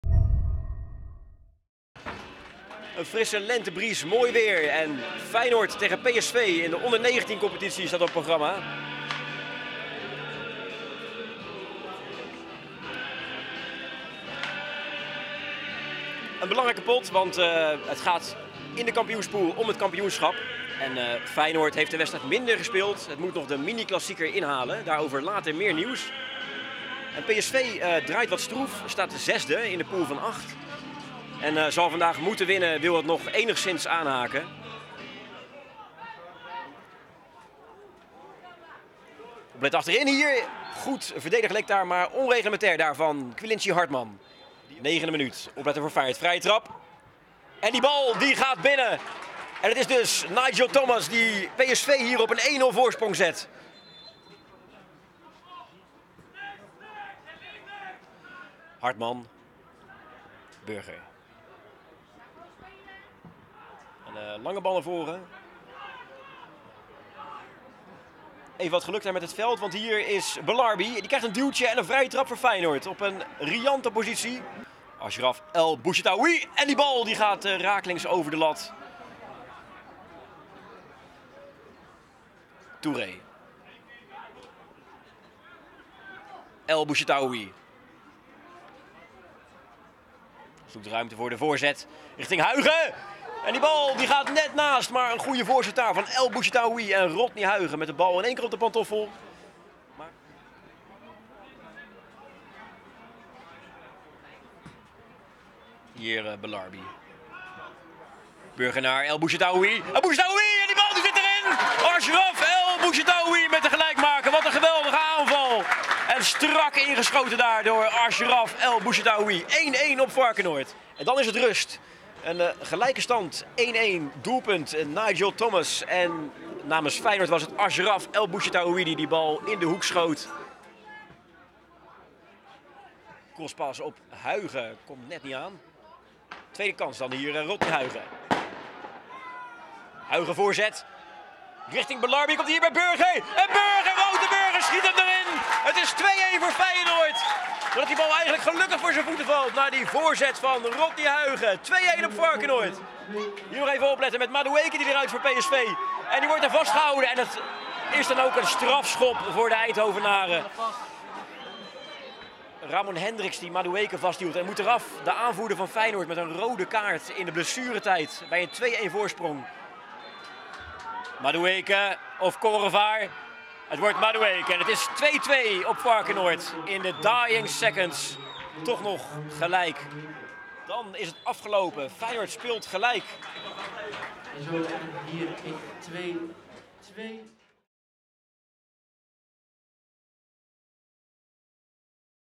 Samenvatting | Feyenoord O19 - PSV O19
Feyenoord Onder 19 liet zaterdag op uiterst zure wijze een belangrijke overwinning uit de handen  glippen. Samenvatting van het duel vs. PSV O19: